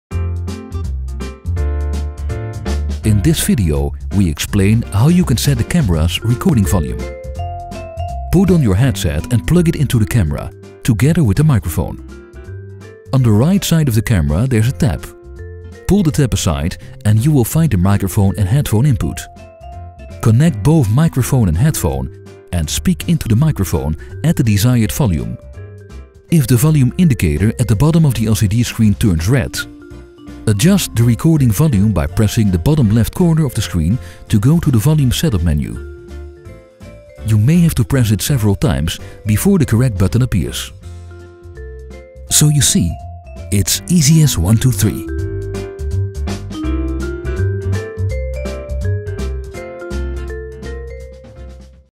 Setting the Recording Volume Panasonic HCX 920
Setting the Recording Volume

In this video we explain how you can set the camera’s recording volume of the Panasonic HCX 920.

Put on your headset and plug it into the camera, together with the microphone.

On the right side of the camera there is a tab. Pull the tab aside and you will find the microphone and headphone input. Connect both microphone and headphone and speak into the microphone at the desired volume.

If the volume indicator at the bottom of the LCD screen turns red, adjust the recording volume by pressing the bottom left corner of the screen to go to the volume setup menu.
You may have to press it several times before the correct button appears.
You see? It’s easy as 1, 2, 3!